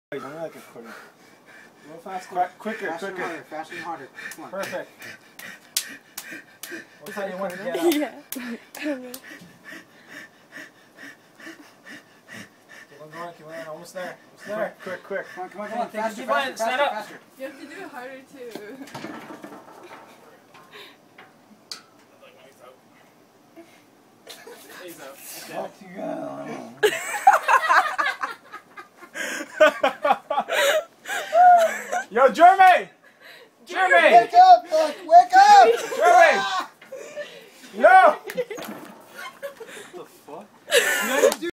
Jeremy Blacking out
Trying to say "fuck you guys" before he passes out lmfao !